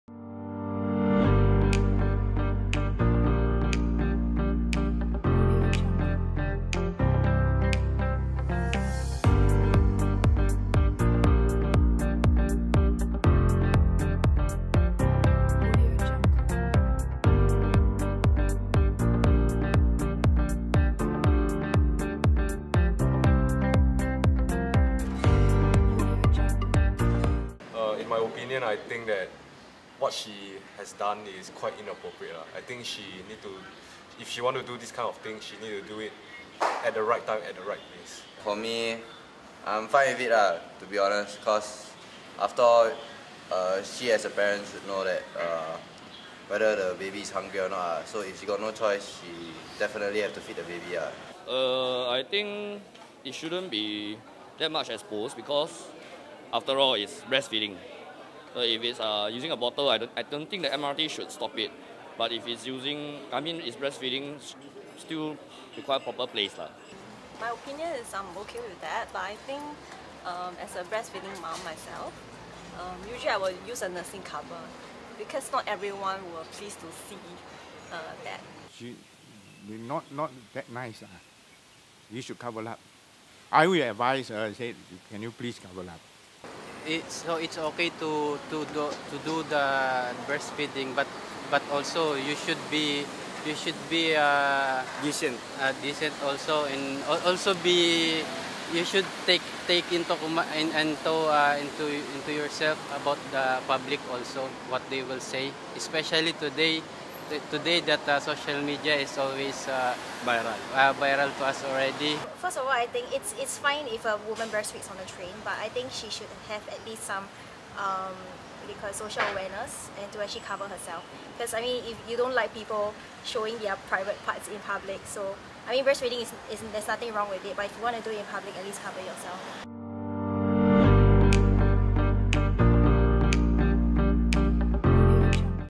Breastfeeding on the MRT: Heartlanders tell Stomp what they think
A photo of a mum breastfeeding her child on the MRT has been circulating on social media, sparking heated debate over whether she should have fed her child in such a public space.